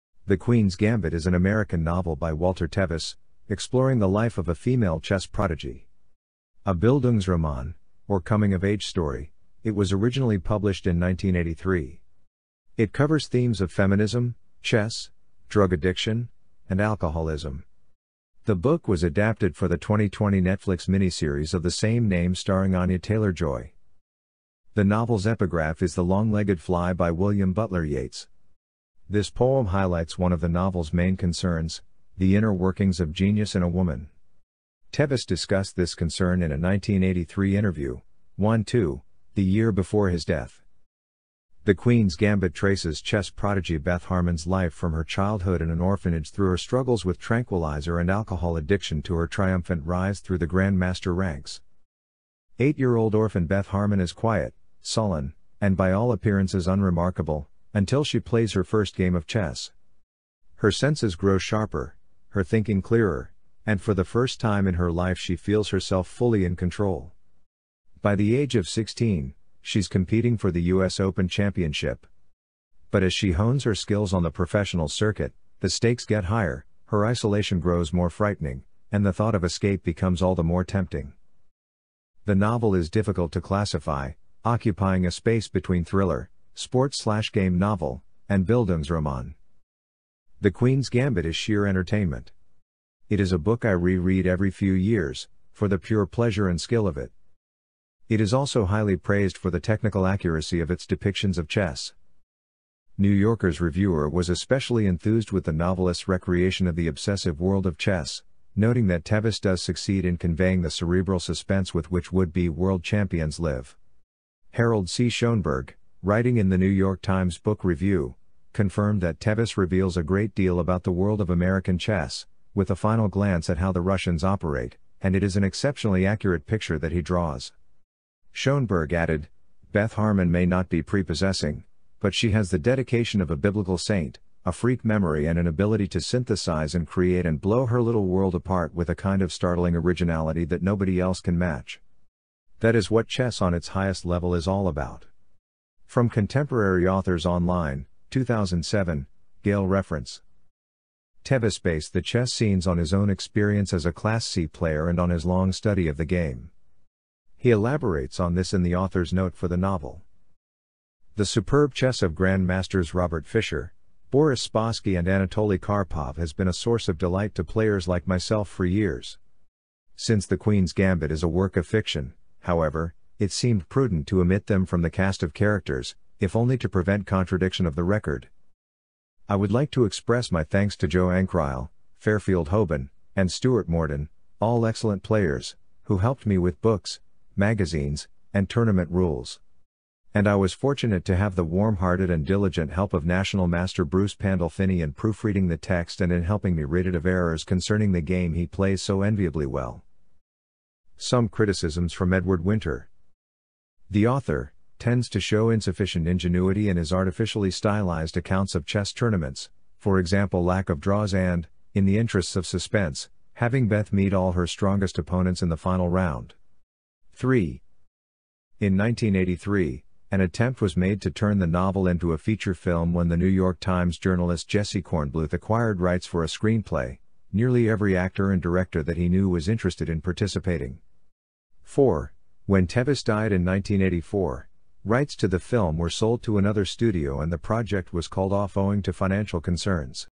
The Queen's Gambit (novel) [WikiThings EN]
The Queen's Gambit is an American novel by Walter Tevis, exploring the life of a female chess prodigy. A bildungsroman, or coming of age story, it was originally published in 1983. It covers themes of adoption, feminism, chess, drug addiction, and alcoholism. The book was adapted for the 2020 Netflix miniseries of the same name.

The novel's epigraph is "The Long-Legged Fly" by William Butler Yeats. This poem highlights one of the novel's main concerns: the inner workings of genius in a woman. Tevis discussed this concern in a 1983 interview,[1][2] the year before his death.

The Queen's Gambit traces chess prodigy Beth Harmon's life from her childhood in an orphanage through her struggles with tranquilizer and alcohol addiction to her triumphant rise through the Grandmaster ranks.

Eight-year-old orphan Beth Harmon is quiet, sullen, and by all appearances unremarkable—until she plays her first game of chess. Her senses grow sharper, her thinking clearer, and for the first time in her life she feels herself fully in control. By the age of sixteen, she's competing for the U.S. Open championship. But as she hones her skills on the professional circuit, the stakes get higher, her isolation grows more frightening, and the thought of escape becomes all the more tempting.

The novel is difficult to classify, occupying a space between thriller, sports/game novel, and bildungsroman.